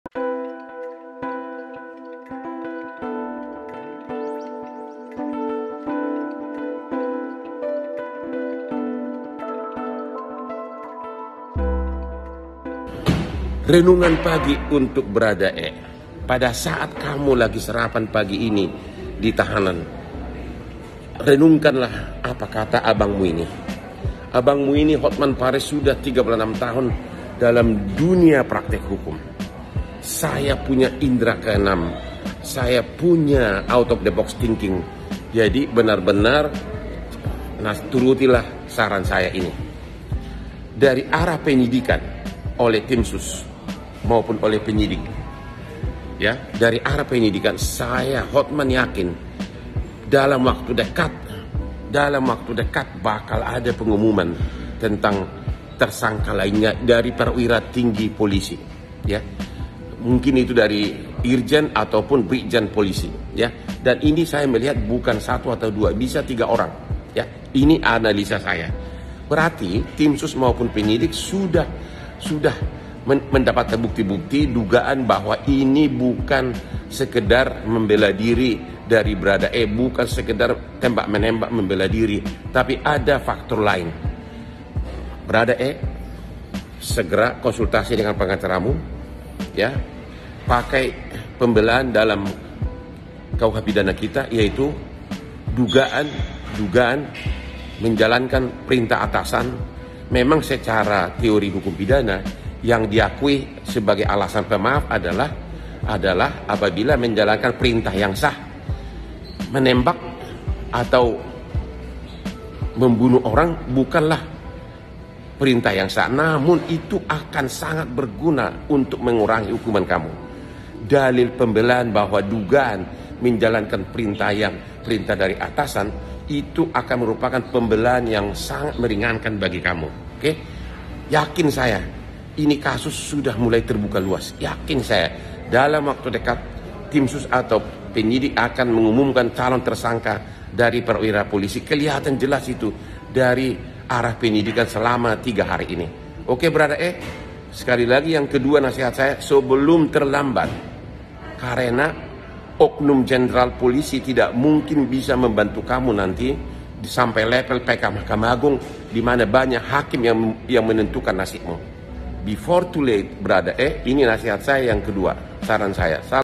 Hotman Paris Sebut Barada E Bisa Bebas?
Pengacara kondang Hotman Paris Hutapea mengomentari kasus pembunuhan Brigadir J.
Menurutnya, hukuman Bharada E bisa ringan jika perintah menghabisi Yoshua adalah perintah resmi dari Irjen Ferdy Sambo